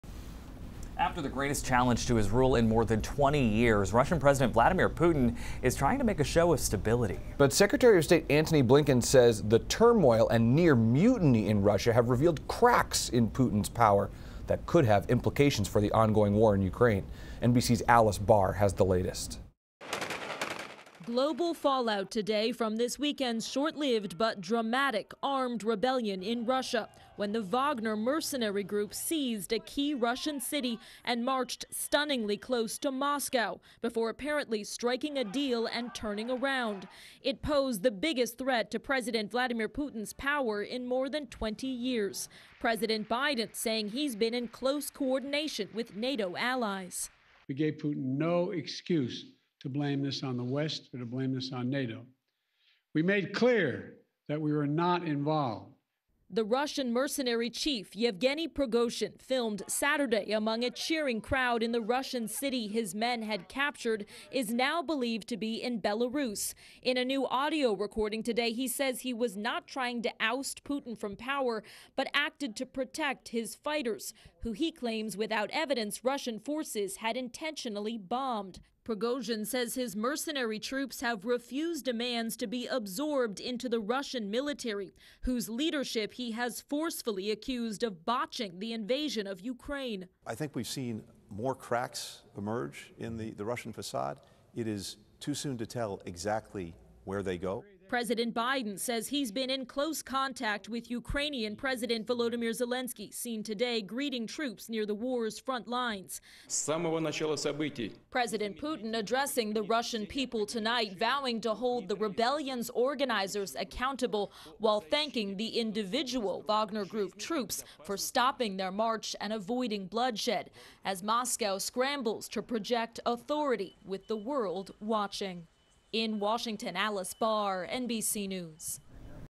Putin attempts to make show of stability after greatest challenge to his rule
Secretary of State Antony Blinken says the turmoil and near mutiny in Russia have revealed "cracks" in Putin's power.